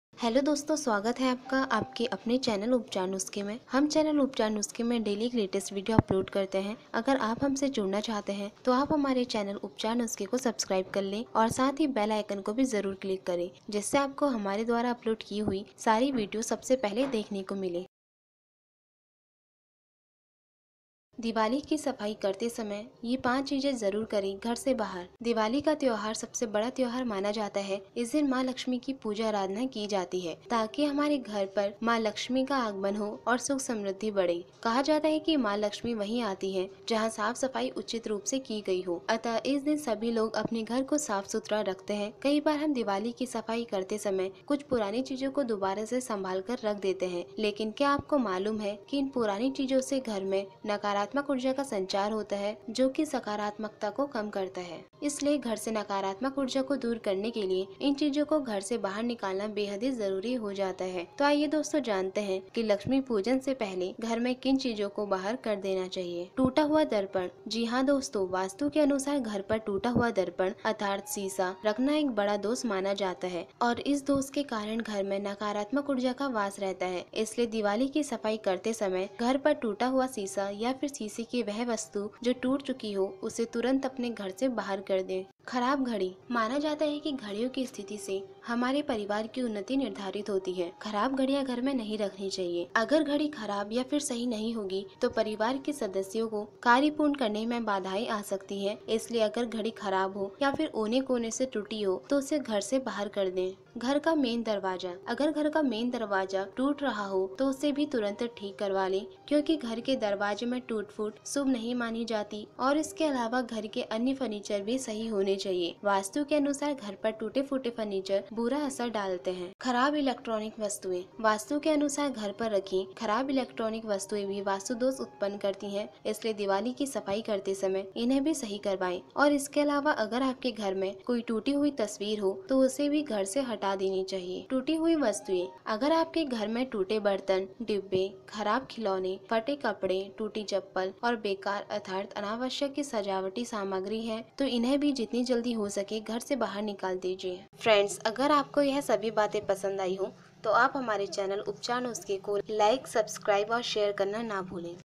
19 October Diwali Cleaning Tips 2017 दीवाली की सफाई करते समय ये 5 चीजें घर से जरूर निकाले
Diwali is considered to be the biggest festival. On this day, Lord Lakshmi is worshiped so that the arrival of Maa Lakshmi at our house and happiness prosperity increases. It is said that Mother Lakshmi comes only where cleanliness is done properly. So, on this day all people keep their house neat and clean. In this video we will tell you that how to clean home on 19 October 2017 Diwali in Hindi.
दीवाली सबसे बड़ा त्यौहार माना जाता है। कहा जाता है कि माँ लक्ष्मी वहीं आती है जहाँ साफ सफाई उचित रूप से की गई हो। अतः सभी इस दिन सभी लोग अपने घर को  साफ़ सुथरा रखते है. कई बार हम दीवाली की सफली करते समय कुछ पुरानी चीजों को दुबारा से संभाल कर रख देते है. लेकिन क्या आपको मालूम है कि इन पुरानी चीजों से घर में नकारात्मक ऊर्जा का संचार करता है जोकि सकारात्मकता को कम करता है। इसलिए घर से नकारात्मक ऊर्जा को दूर करने के लिए इन चीजों को घर से बाहर निकालना बेहद ही जरूरी हो जाता है तो आईये जानते है कि लक्ष्मी पूजन से पहले घर में किन चीजों को बाहर कर देना चाहिए.